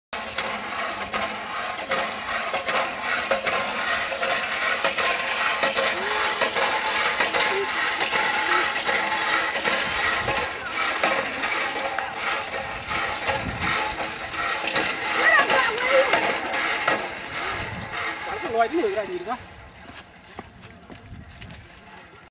Water Pump in Action
Girls and women in South Sudan spend much of their time each day collecting the water for their families. This clip shows a hand pump in action as girls fill their jerricans in the village of Wunlang.